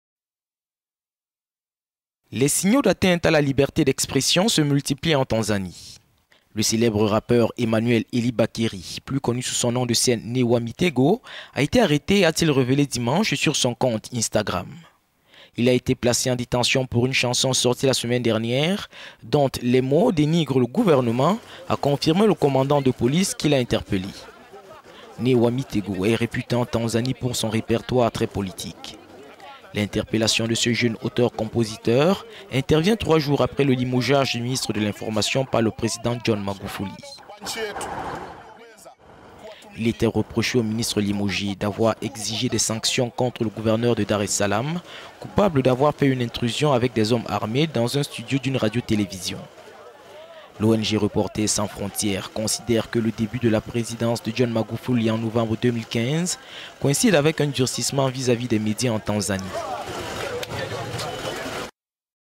Tanzanie : arrestation d'un célèbre rappeur pour avoir "dénigré" le gouvernement
Un célèbre rappeur tanzanien a annoncé avoir été arrêté dimanche par la police à cause de l'une de ses chansons critiquant le pouvoir du président John Magufuli.

"C’est la vérité, je suis aux arrêts. En ce moment, je suis en train d’être emmené au commissariat de police de Mvomero. J’aime tous les Tanzaniens", a écrit sur son compte Instagram Emmanuel Elibariki, connu sous le nom "Nay wa Mitego" et dont les vidéos sur YouTube sont vues par des milliers de fans.